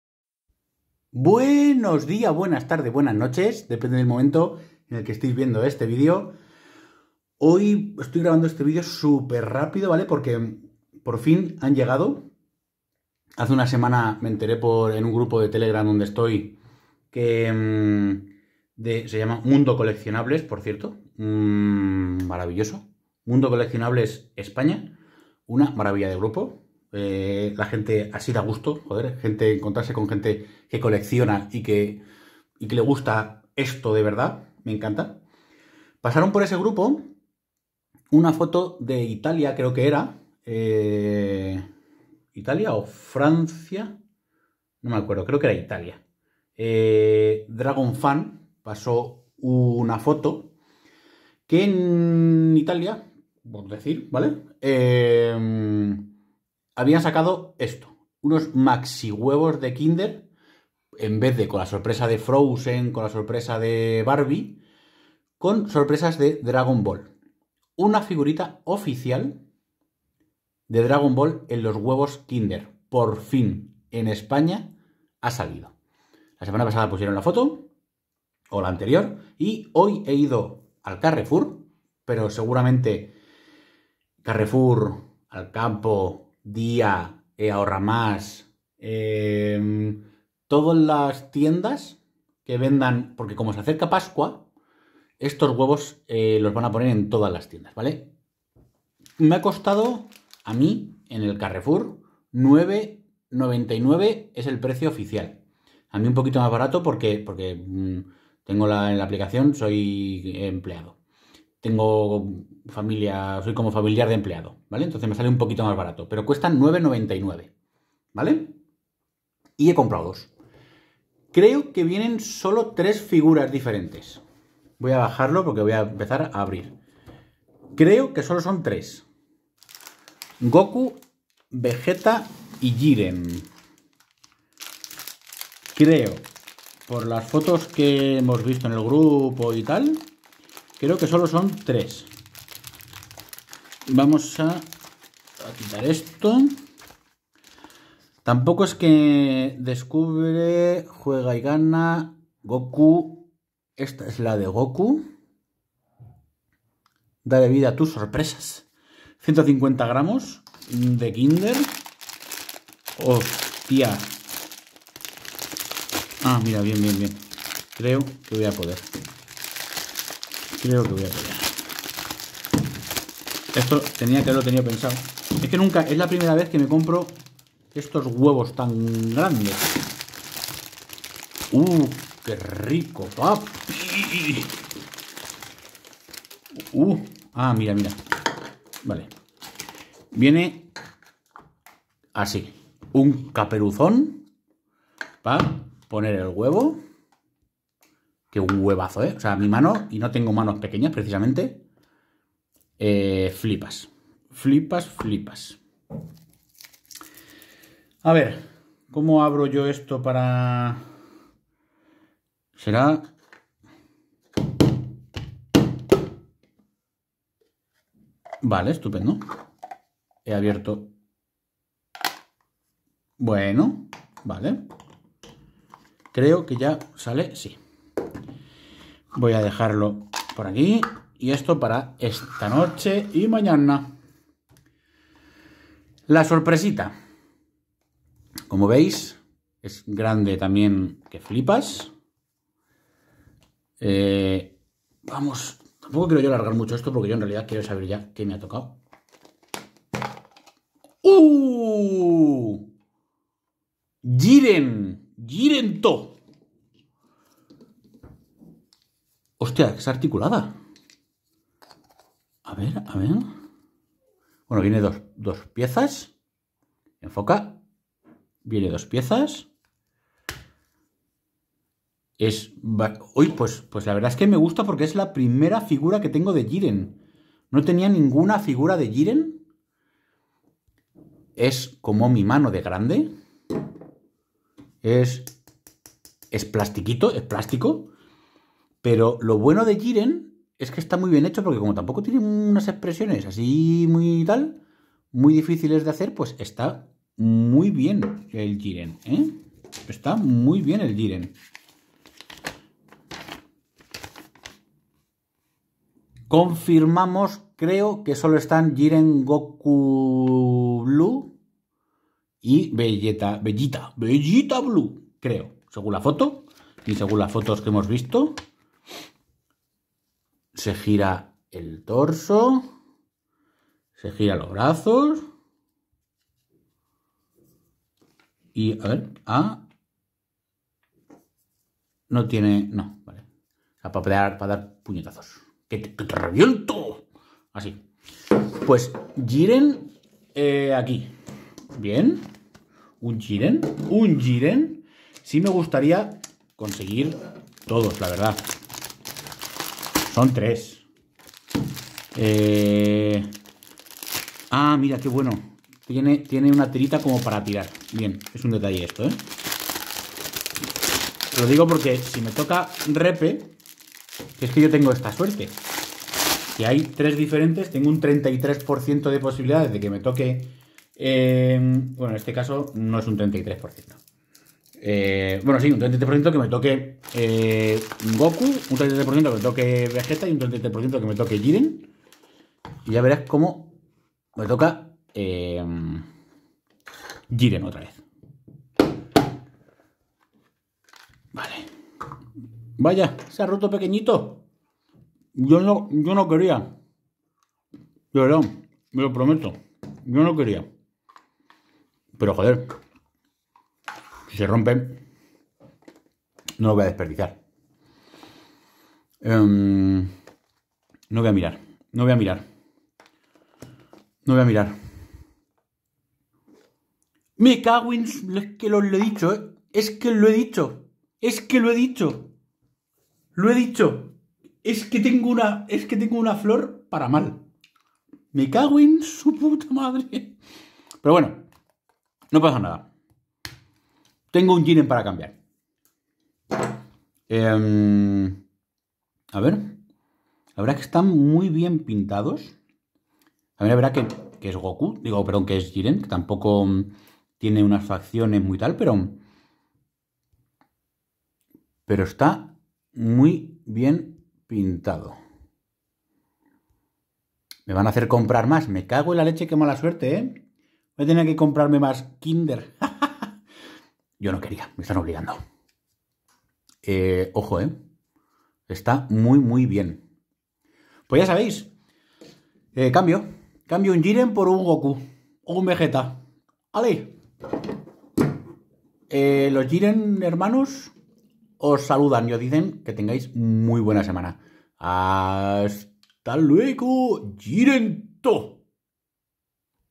LLEGA OFICIALMENTE DRAGON BALL A LOS KINDER CON 3 SUPER FIGURAS DE GRAN TAMAÑO. SON ENORMES
Cualquier duda, pregunta o sugerencia, no dudéis en ponerlo en los comentarios.